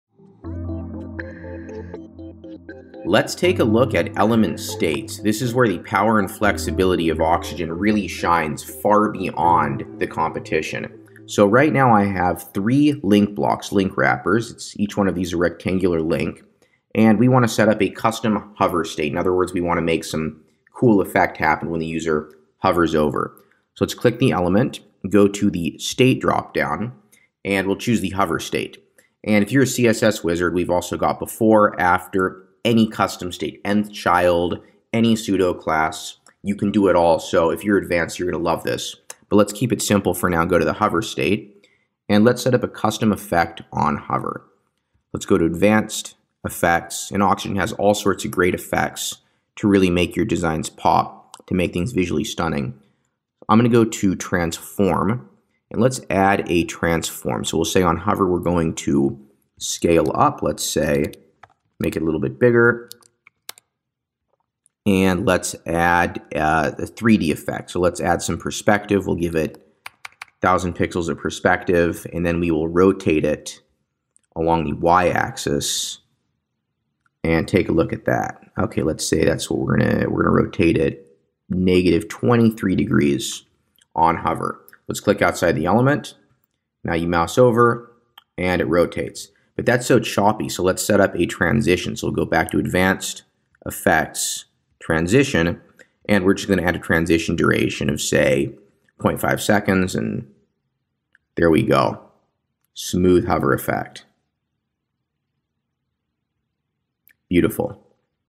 Oxygen's States - Awesome Hover Effects
Oxygen's ability to visually style states like :hover lets you quickly create incredible hover effects for any element.